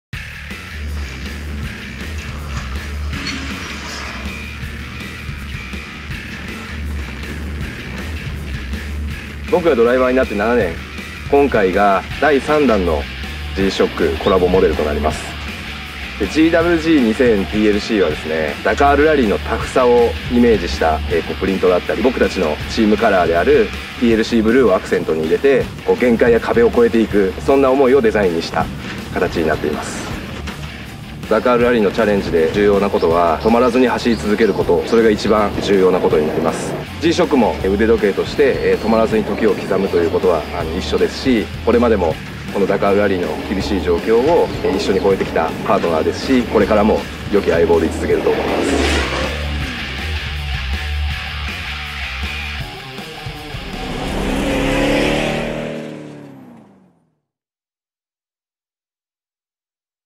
GWG-2000TLC｜Collaboration Toyota Land Cruiser | Casio G-Shock
Modelo de colaboración TEAM LAND CRUISER TOYOTA AUTO BODY.
Se trata del tercer modelo de colaboración "Team Land Cruiser Toyota Auto Body" y G-SHOCK que sigue evolucionando en busca de la resistencia "Rally" que atraviesa todas las superficies de carreteras, desde desiertos hasta zonas embarradas y montañosas.
Un conmemorativo modelo de colaboración de G-SHOCK que apoyó al "Team Land Cruiser" que ha participado en el "Rally Dakar", que es conocido como el evento más duro del mundo, y demostró su dureza junto con el Land Cruiser.
Diseñado con la idea de ir más allá de los límites y las paredes con el color del equipo "TLC Blue" como acento.